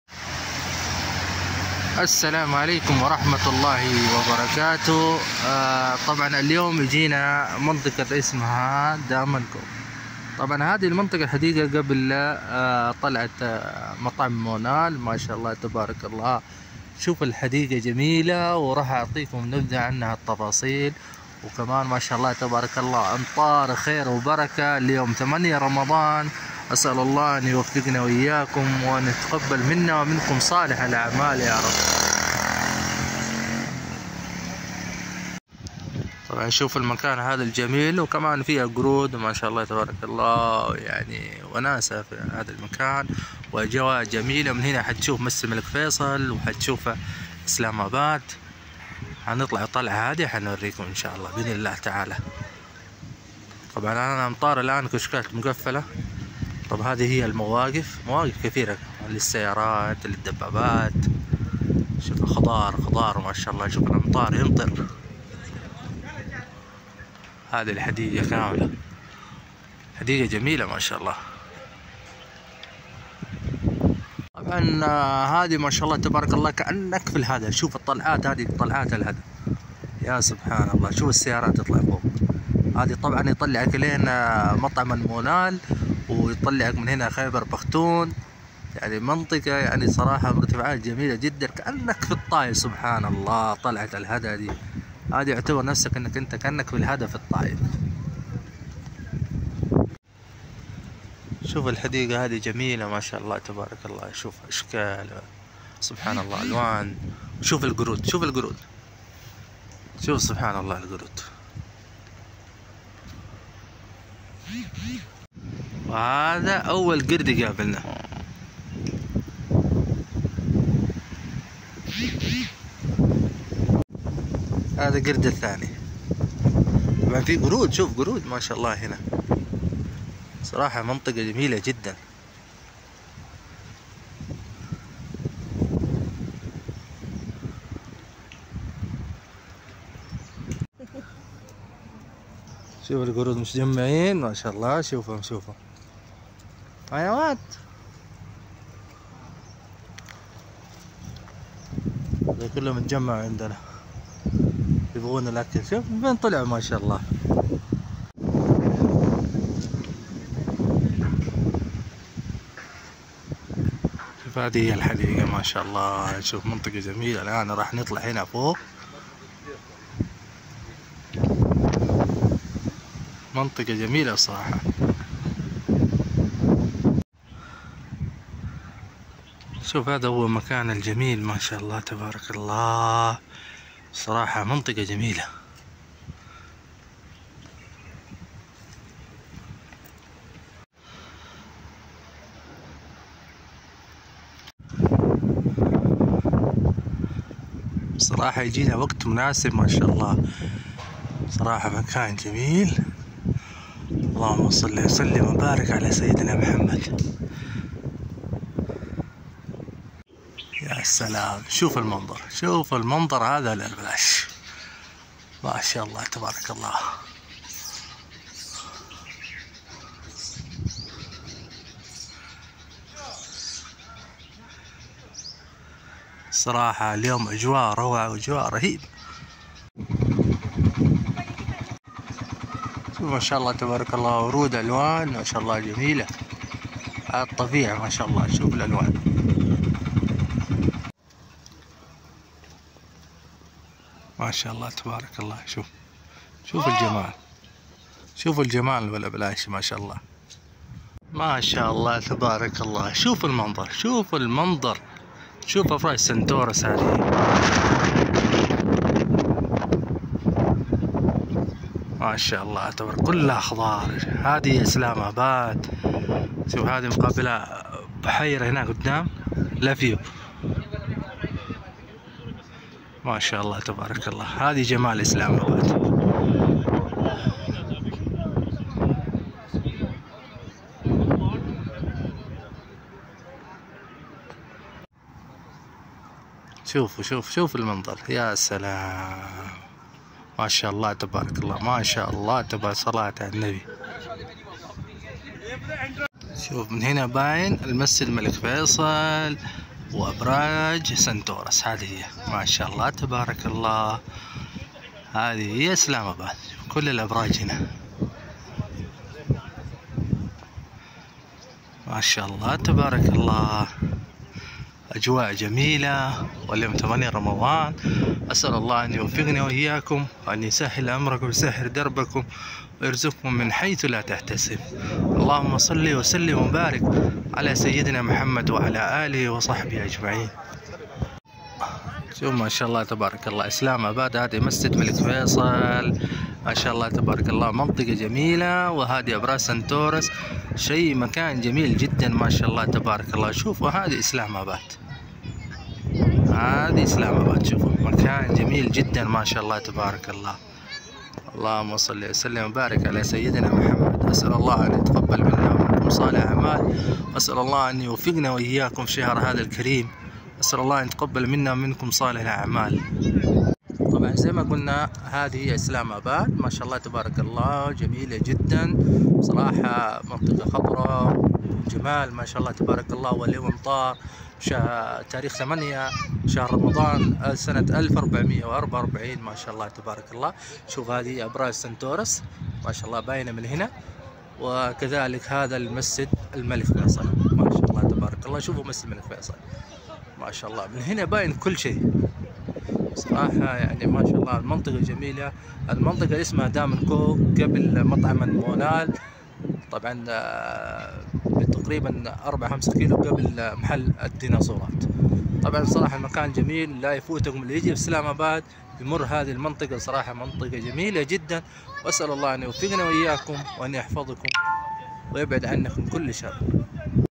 أجمل حديقة في إسلام أباد دامن كوه Daman-e-koh islamabad
حديقة أعلى تل شمال إسلام أباد وتقع في وسط تلال مارجالا. اسمها عبارة عن اقتران من كلمتين فارسيتين ، والتي تعني معًا تلال القدم. فهو يقع في حوالي

2400 قدم من مستوى سطح البحر وحوالي 500 قدم من مدينة إسلام أباد. إنه المكان مشهور

حديقة دامن كوه إسلام أباد وتشوف منظر إسلام آباد حديقة جميلة وروعه هذه قبل مطعم مونال 4 كيلو حديقة فيها جمال ورود طبيعة ونظافة واهتمام وفيها جلسات وفي أعلى مكان يبان منظر اسلام آباد ومسجد الملك فيصل وأبراج سنتورس مكان جميله  وفيها قرود كانك في الطائف الهدا أجواء جميلة وأتمني اللي يجي إسلام أباد يمر هذا المكان
8 رمضان 1444

جمال باكستان